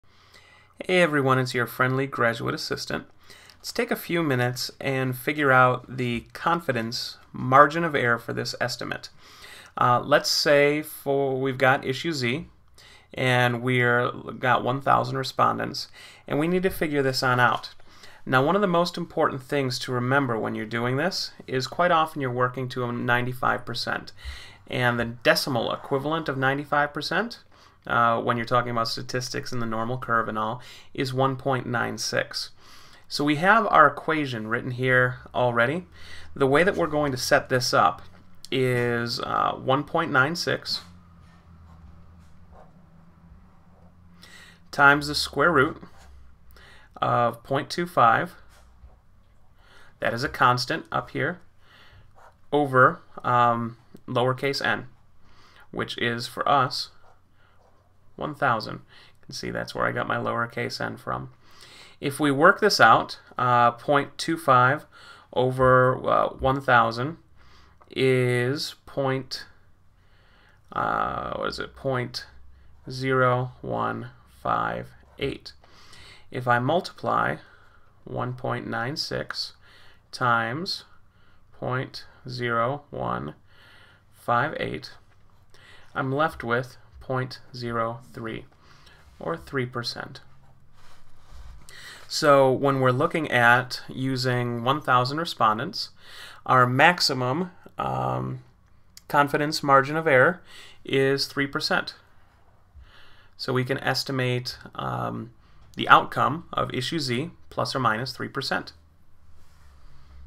95% confidence margin of error
This video will help you to figure out the confidence margin of error to 95%